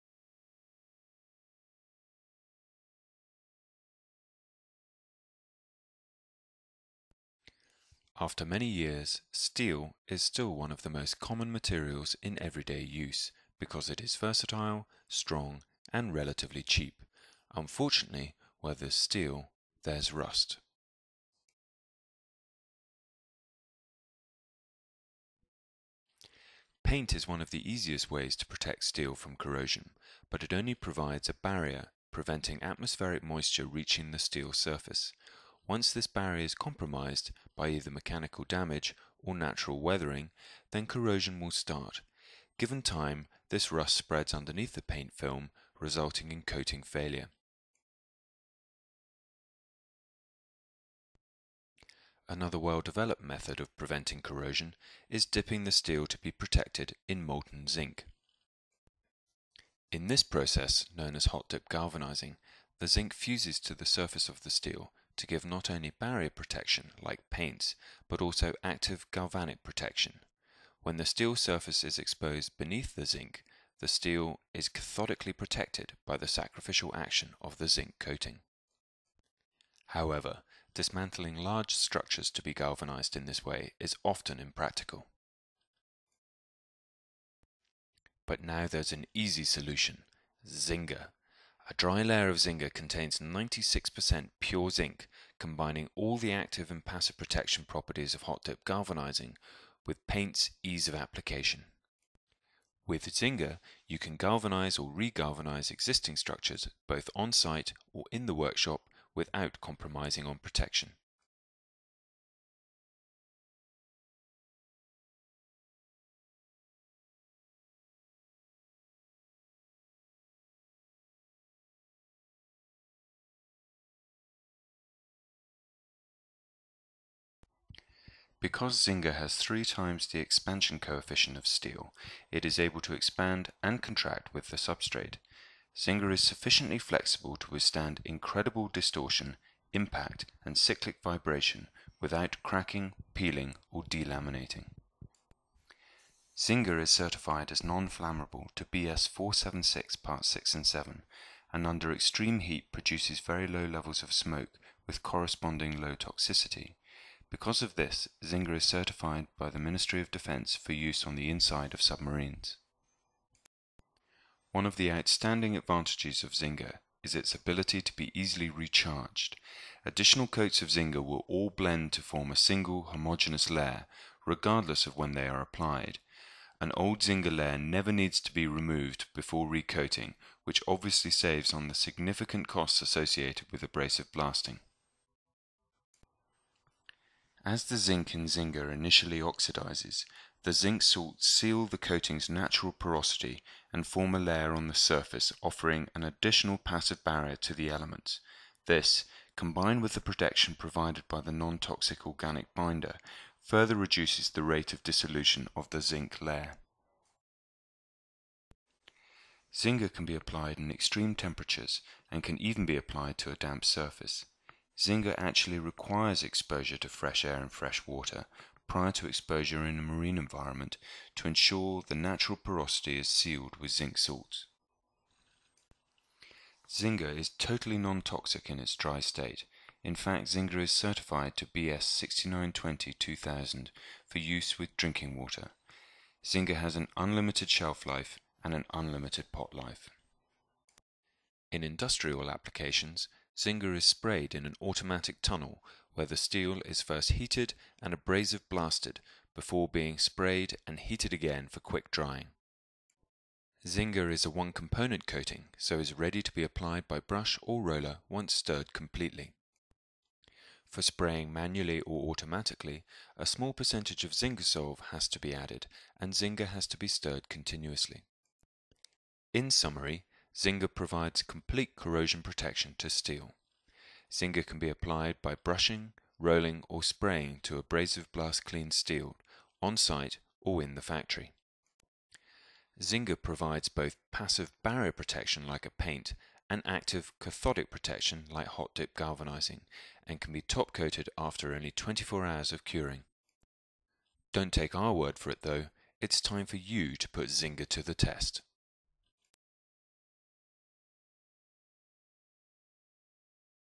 Zinga Cathodic Protection = NORSOK Approved Standard M-501
This proves incredible ability to prevent corrosion and cannot be compared to any other conventional paint system - it is truly uniqe !